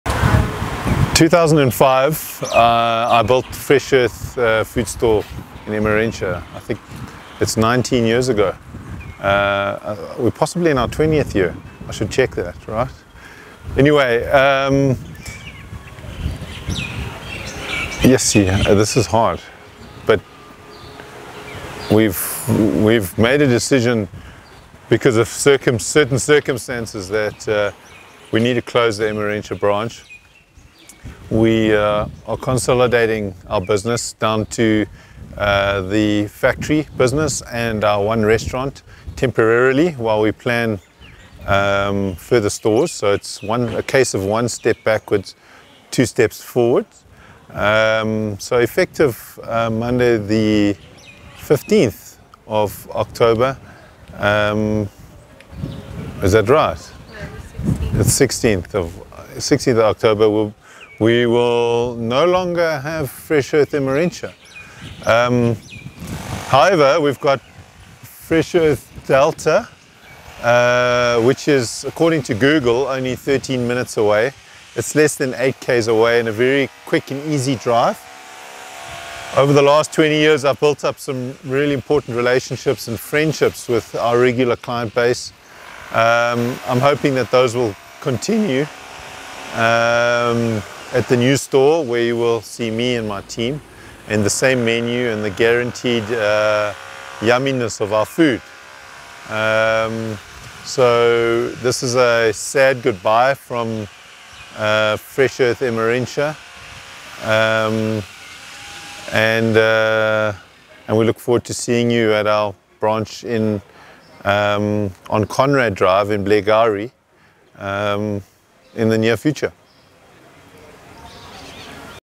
Goodbye Fresh Earth Emmarentia 2022
Goodbye Emmarentia. We've learned, we've loved, we've laughed. It's been quite a journey since 2005 when we first opened our doors. So many faces, interactions and a beautiful community created.

From scraping together what we have to get it off the ground, to running queues on a Saturday around the block, all our Heritage day bashes, working through COVID and now winding down the end of an extraordinary experience in hopes of a future adventure.

Our doors will sadly close on Sunday 16 October 2022 at 3pm. Thereafter you can find us at Delta which is only 13min away - 74 Hillcrest Ave, Blairgowrie and you can still shop online at freshearth.co.za.

My crew and I hope to still see you all in store at Delta for more fun and memorable moments.

Matthew & The Fresh Earth Emmarentia Team

(P.S. Excuse the inaccurate dates mentioned in the video - it was really a hard one to film).